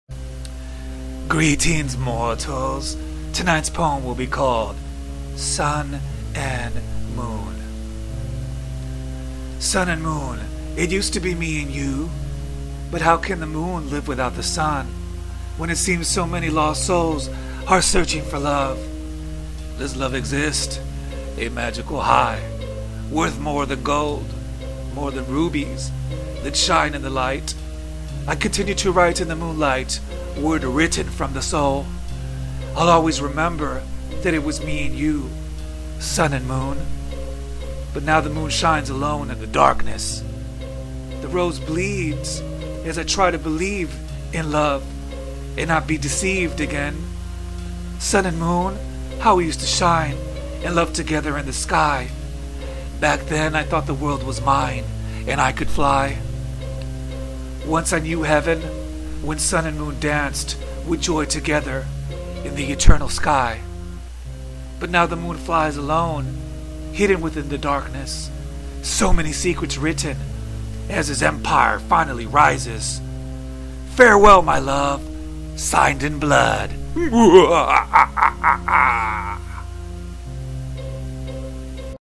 Sun and Moon Spoken Word Dark Love Poetry
Sun and Moon is a dark spoken word love poetry piece. This is a very good poem.